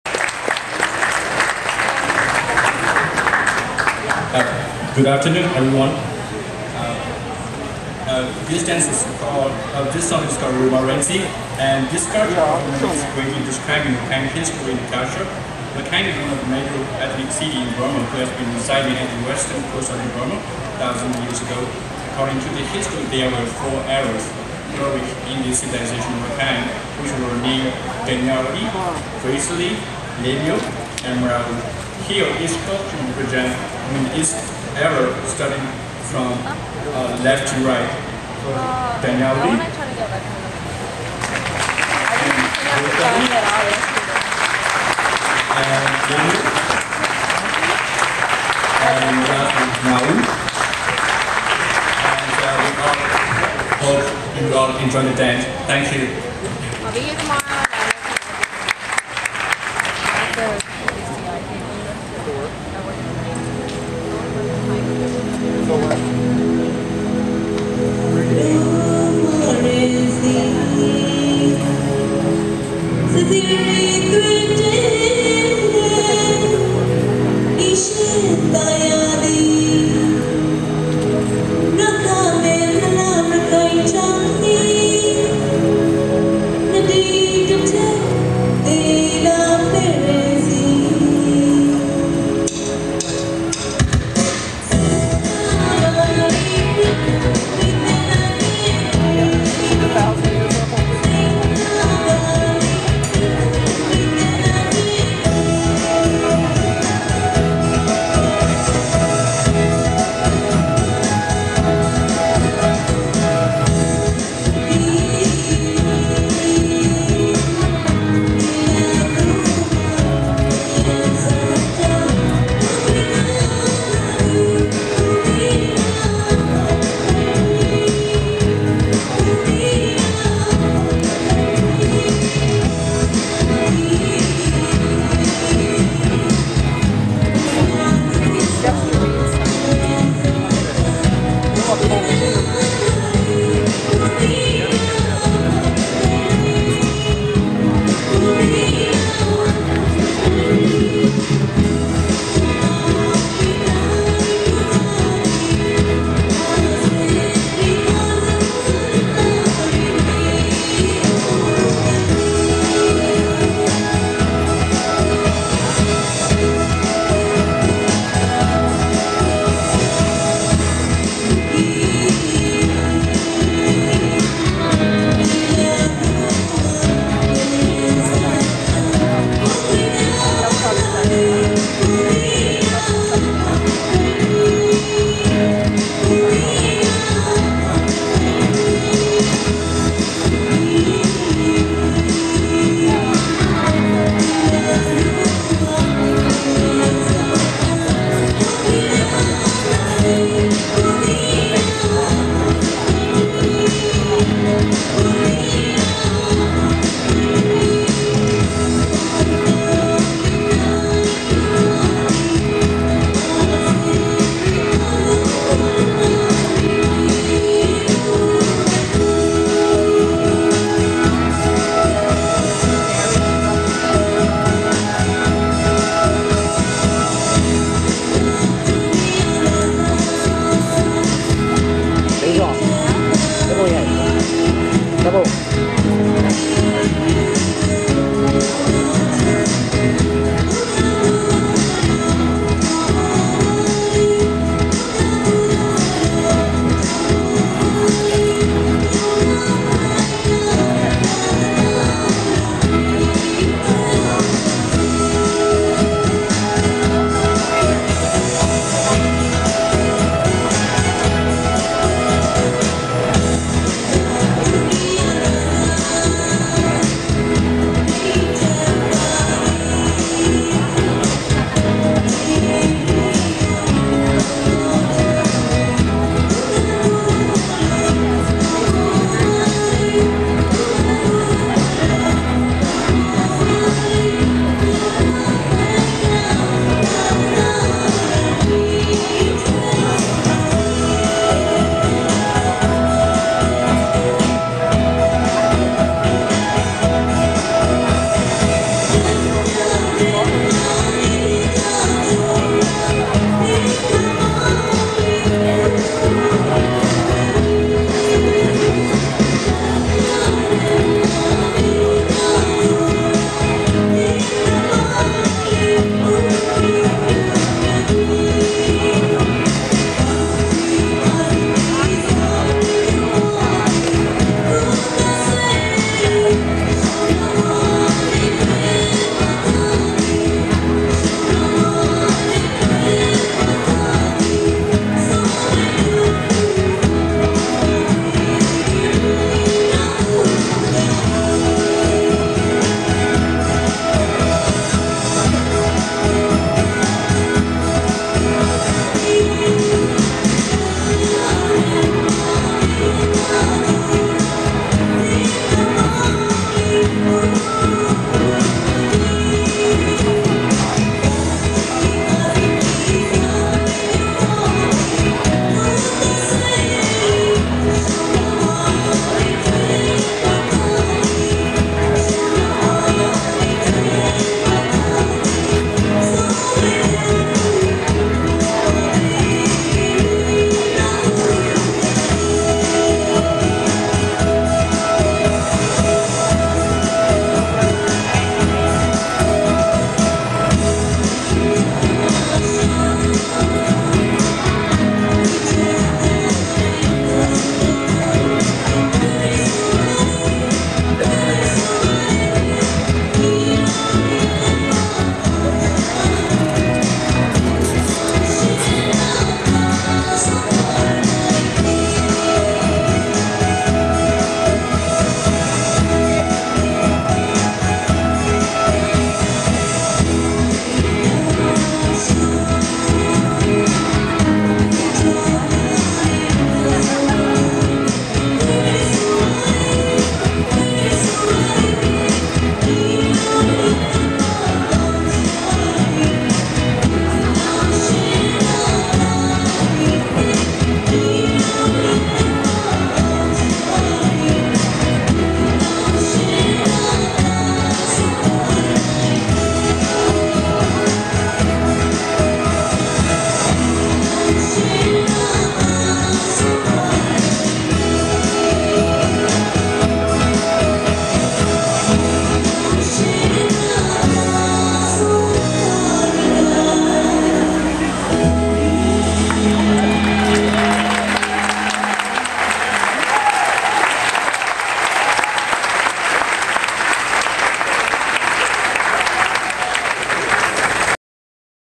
rakhine dance @smithsonian folklife festival ,washington D.C ,USA
2010 Smithsonian Folklife festival,Washington D.C,USA performed by UTICA RAKHINE FAMILY,NY